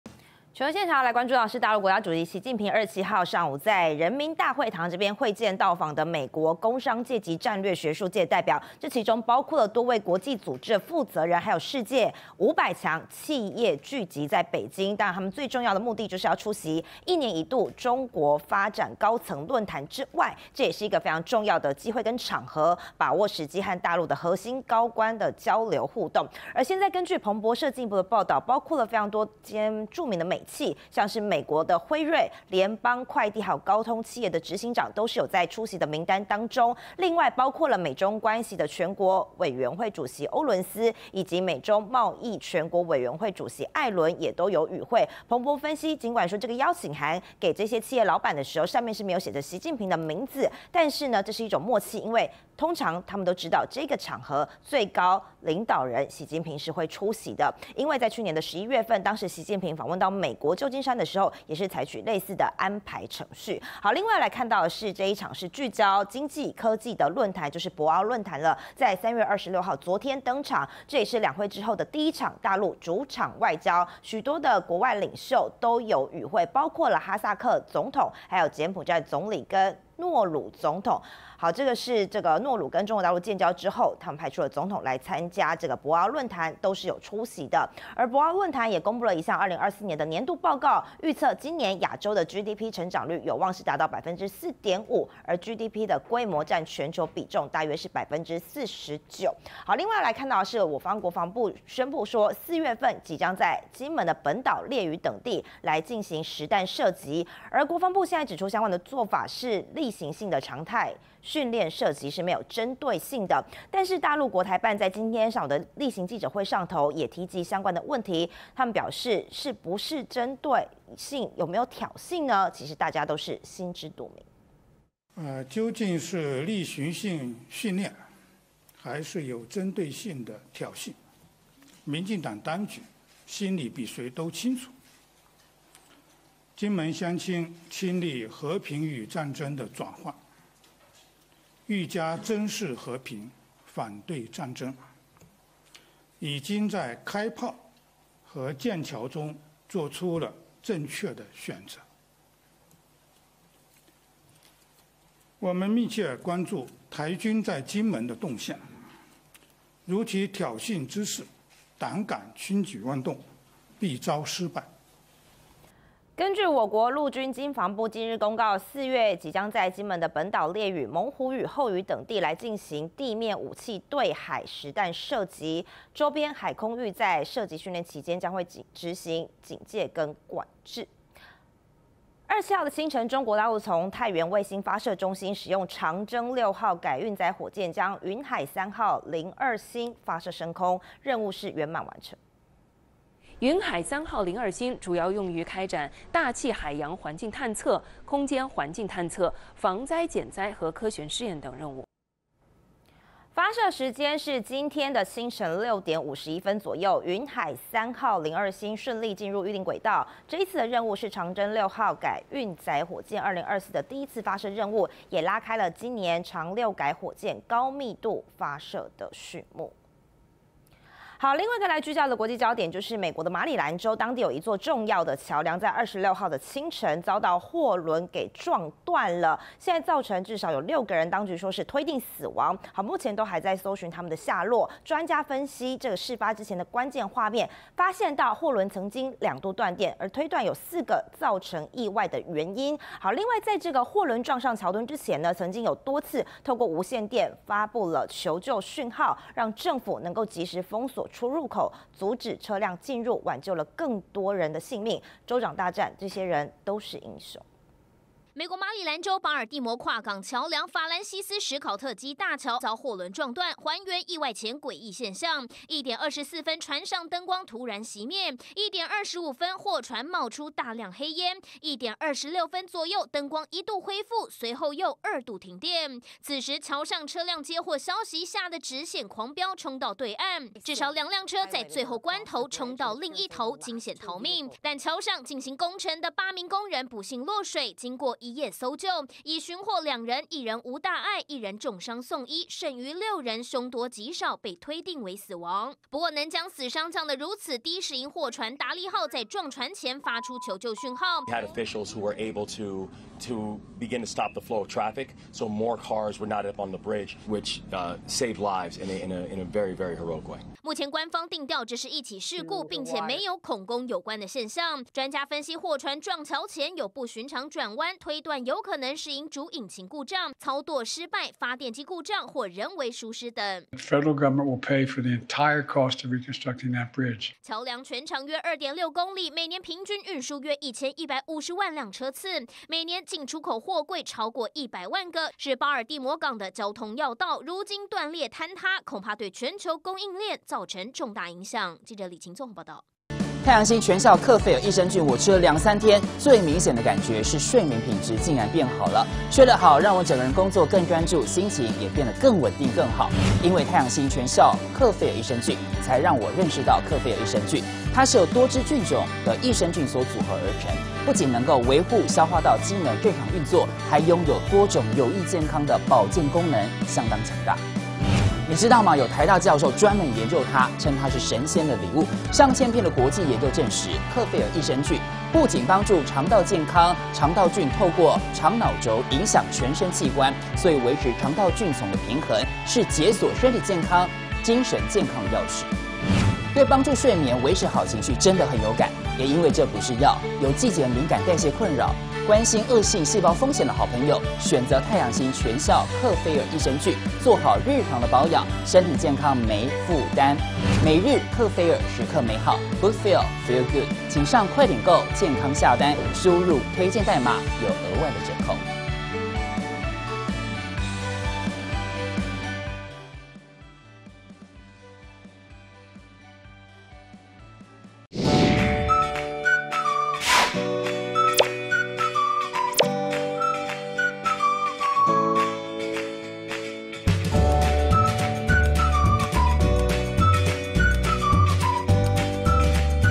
【每日必看】習近平將會晤美商會領袖 大老闆名單曝｜中美商界交流 習近平.美國工商界和戰略學術界代表會面｜陸"長征六改"火箭2024首飛 成功發射雲海三號02星 20240327
00:00 中美商界交流 習近平.美國工商界和戰略學術界代表會面
00:52 為會見陸高層 世界500強企業齊聚北京
01:08 博鰲亞洲論壇登場 兩會後首場主場外交
01:43 金門4月實彈射擊演練 陸國台辦:輕舉妄動必遭失敗
03:03 陸"長征六改"火箭2024首飛 成功發射雲海三號02星
03:41 空拍慘況曝! 貨輪撞巴爾的摩大橋前"冒煙2度停電"